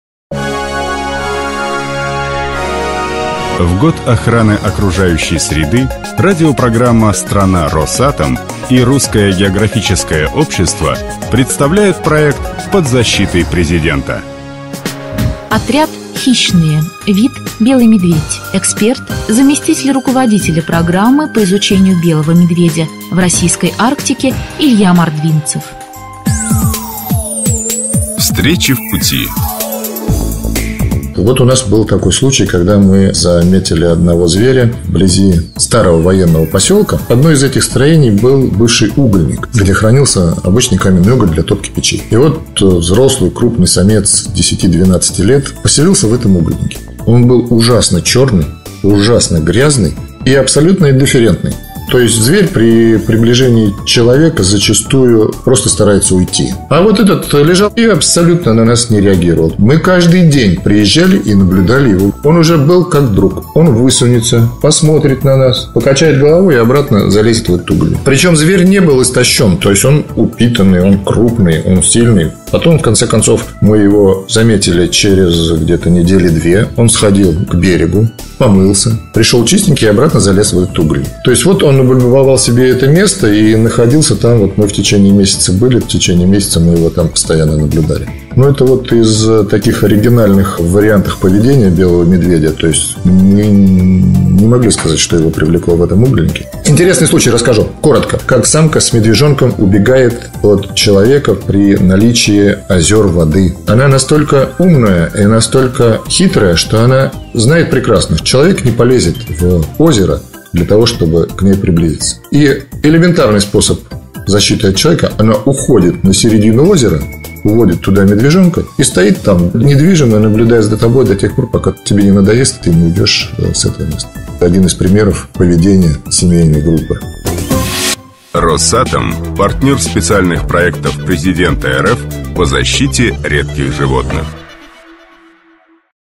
Выпуск №2 Белый медведь
Как белые медведи становятся черными?